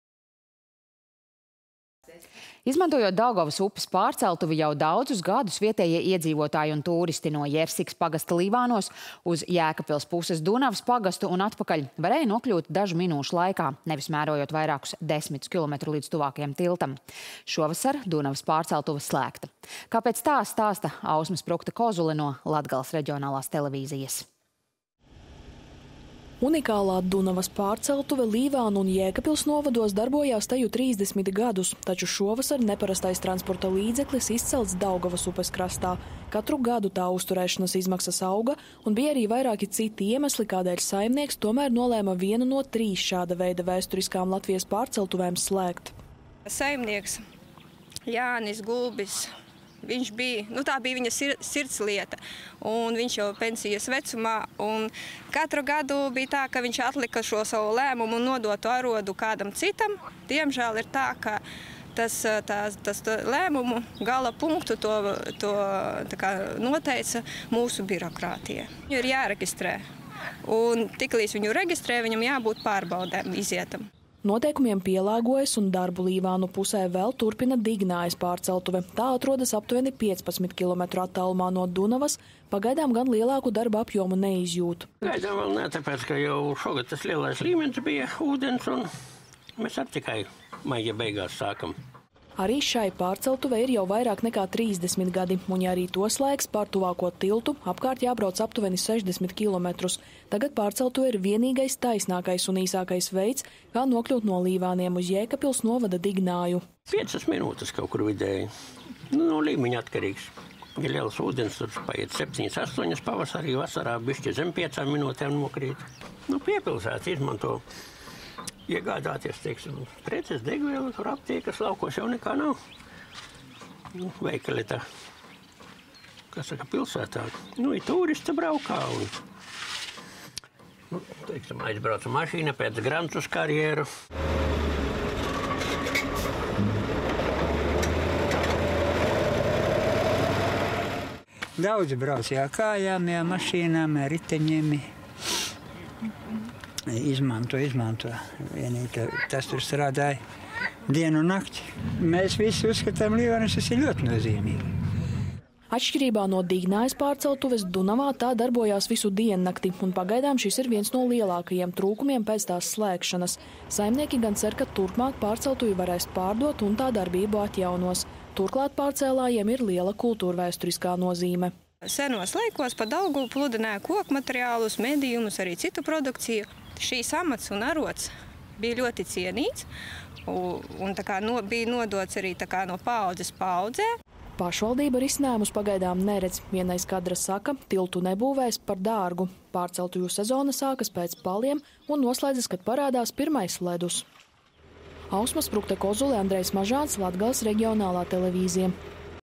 Darbu pārtraukusi sena Daugavas pārceltuve Dunavā
Izmantojot Daugavas pārceltuvi, vietējie iedzīvotāji un tūristi no Jersikas pagasta Līvānos uz Jēkabpils puses Dunavas pagastu, kā arī atpakaļ varēja nokļūt dažu minūšu laikā, nevis mērojot vairākus desmitus kilometru līdz tuvākajam tiltam. Šovasar Dunavas pārceltuve ir slēgta. Turpina Ausma Sprukte-Kozule no Latgales reģionālās televīzijas.